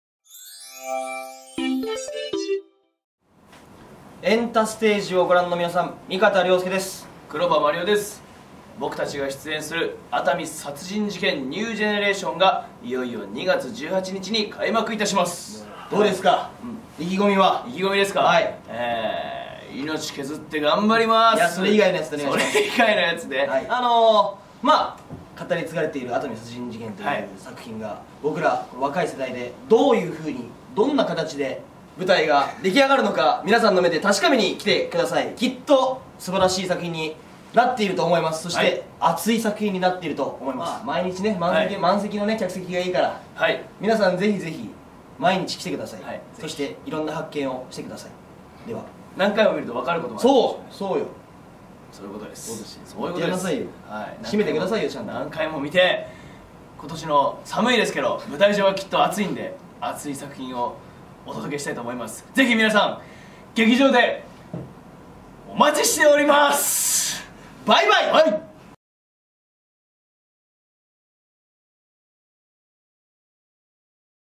『熱海殺人事件 NEW GENERATION』味方良介＆黒羽麻璃央からメッセージ！ | エンタステージ
2017年2月18日（土）より東京・紀伊國屋ホールにて開幕する『熱海殺人事件 NEW GENERATION』。本作は、紀伊國屋ホールにて春の風物詩として幾度となく上演され続けている演劇界の巨匠つかこうへいの代表作です。今年は“NEW GENERATION”と銘打ち、味方良介、文音、多和田秀弥、黒羽麻璃央の4名の若き俳優たちが、新しい『熱海殺人事件』を生み出すべく奮闘しています。本番を間近に控える中、木村伝兵衛部長刑事役の味方と、犯人・大山金太郎役の黒羽からメッセージが届きました！二人の熱い思いを、公開中のインタビューと合わせてお楽しみください。

◆公演情報
『熱海殺人事件 NEW GENERATION』
2017年2月18日（土）～3月6日（月）　東京・紀伊國屋ホール
【作】つかこうへい
【演出】岡村俊一
【出演】味方良介、文音、多和田秀弥、黒羽麻璃央